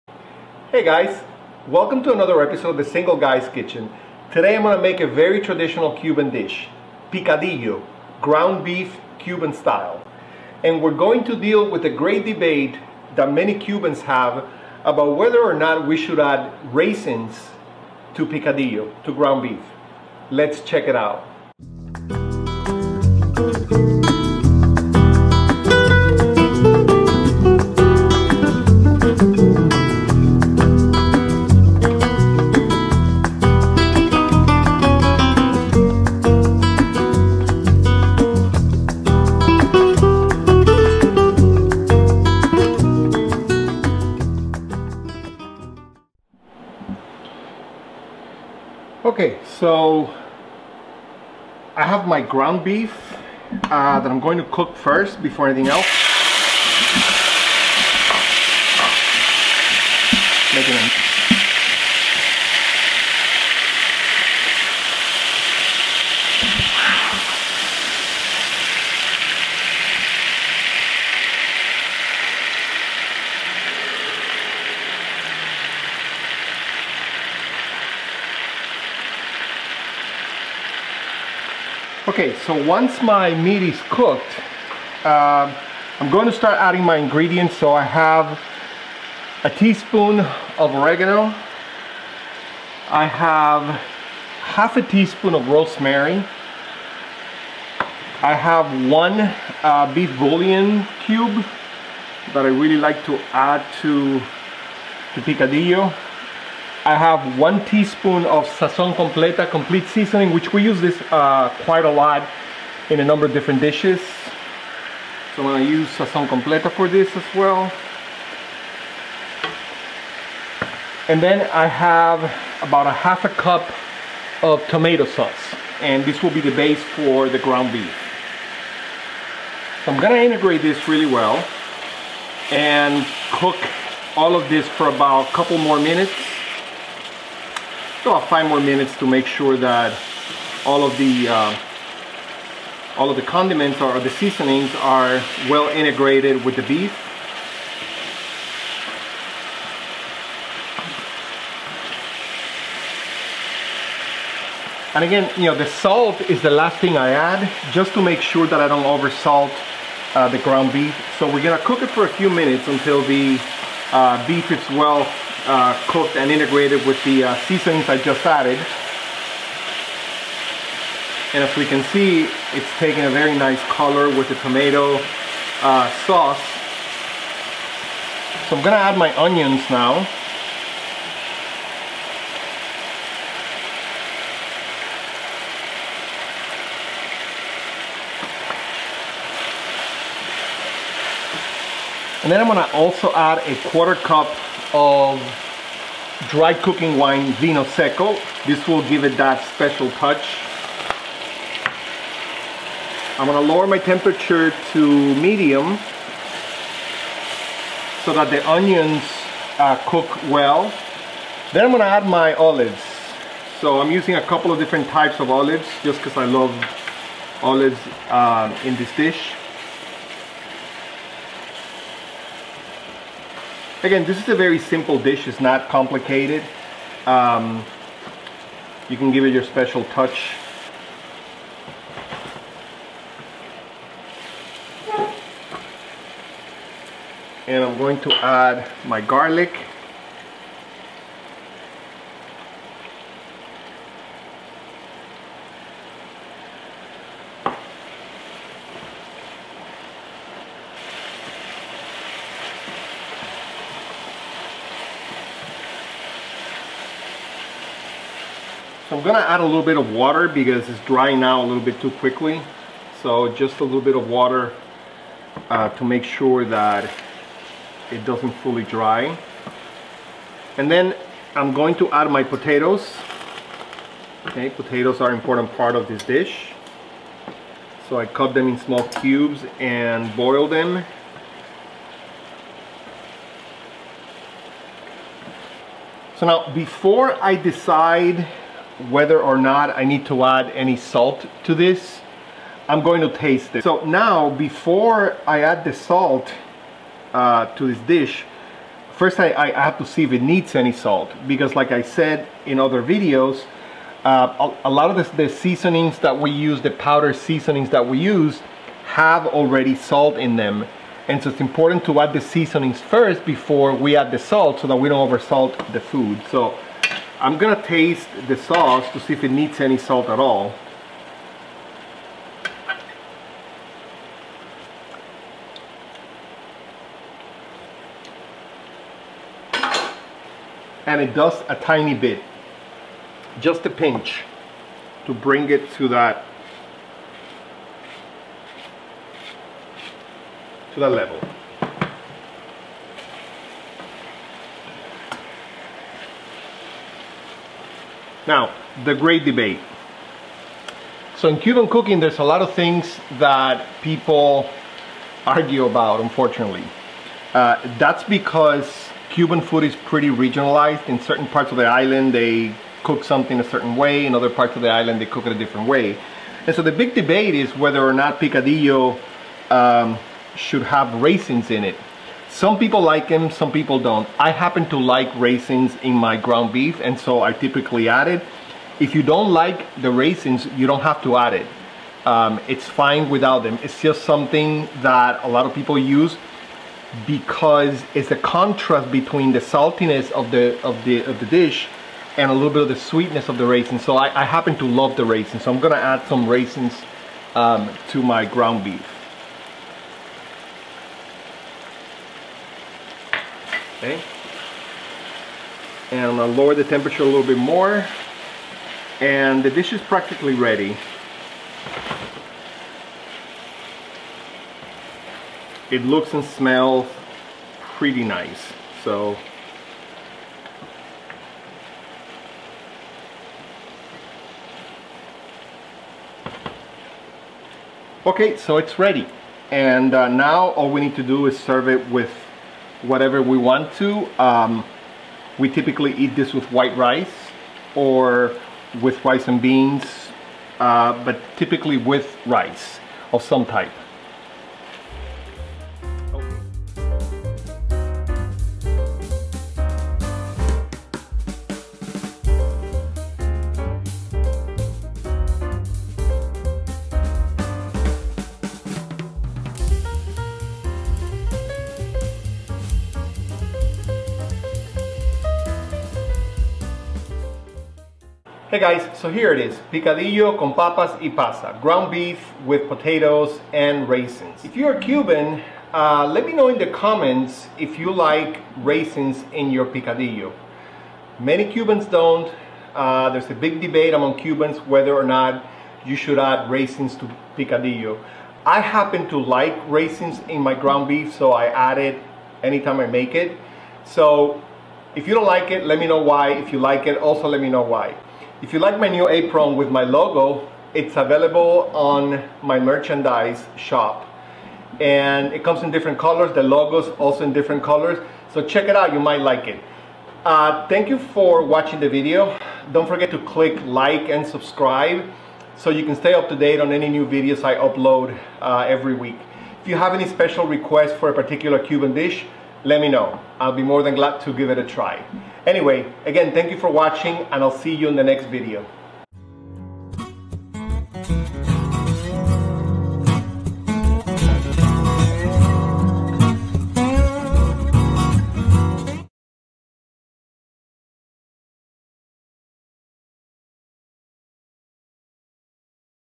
Picadillo con Papas y Pasas- Cuban Ground Beef with Potatoes and Raisins
Picadillo is a very traditional cuban beef dish. But there is a great debate among Cubans of whether or not we should add raisins to it. Some people love some hate it. I happen to like it and if you do please do et me know in the comments. I hope you enjoy this video.